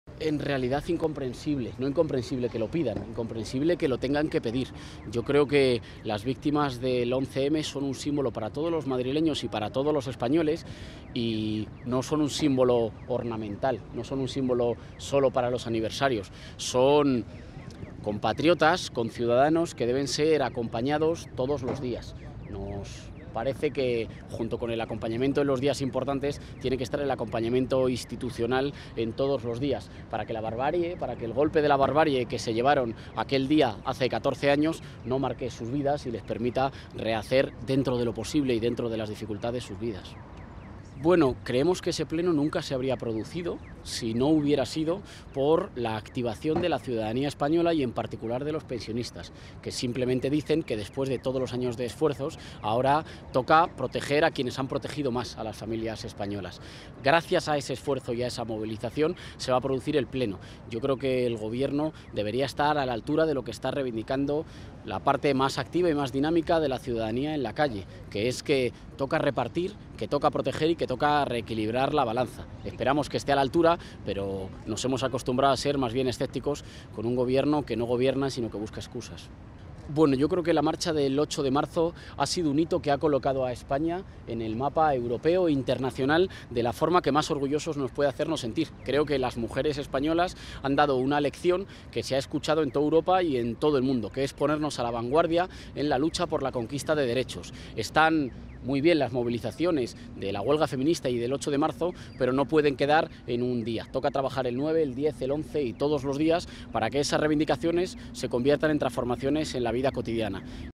Errejón ve "incomprensible" que víctimas tengan que seguir reclamando ayudas
El diputado y secretario de Análisis Estratégico y Cambio Político de Podemos, Íñigo Errejón, ve "incomprensible" que las víctimas del 11-M tengan que seguir reclamando ayudas cuando son "un símbolo".

   "No son un símbolo ornamental, un símbolo sólo para los aniversarios. Son compatriotas, conciudadanos que deben ser acompañados todos los días", ha defendido.